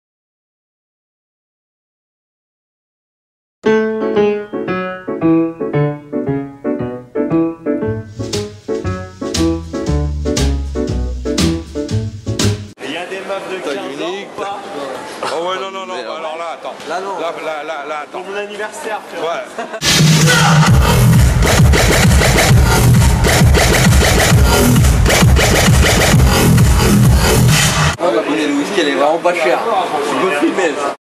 X'CUZE MY FRENCH (Tristan Casara & Nicolas Monier) at Green Mango, Koh Samui (Thailand)
X'CUZE MY FRENCH playing at Green Mango, Koh Samui (Thailand) on Sunday 15th December 2013.